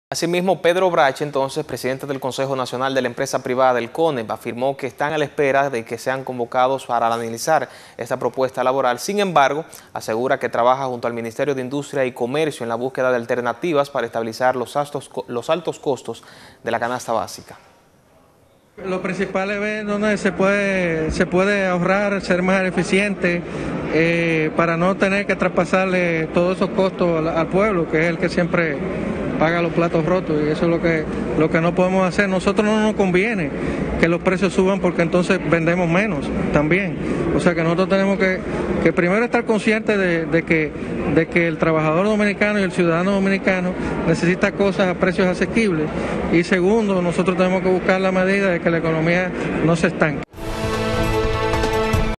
Presidente del CONEP espera conocer propuesta de aumento salarial.
Pedro Brache, presidente del Consejo Nacional de la Empresa Privada (Conep), afirmó que está a la espera de que los convoquen para analizar la propuesta laboral, sin embargo, aseguró que trabajan junto al Ministerio de Industria y Comercio (MICM) en la búsqueda de alternativas para estabilizar los altos costos de la canasta básica.